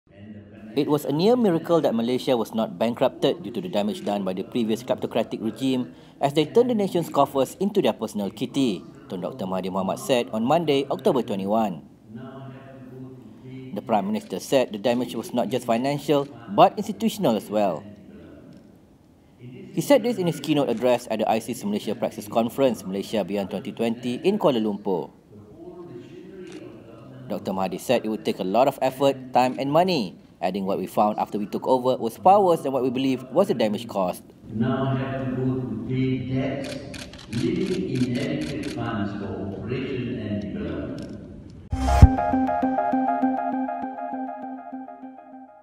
PM: Near miracle Malaysia not bankrupted by 1MDB scandal
It was a near miracle that Malaysia was not bankrupted due to the damage done by the previous kleptocratic regime as they turned the nation’s coffers into their personal kitty, Tun Dr Mahathir Mohamad said on Monday, October 21.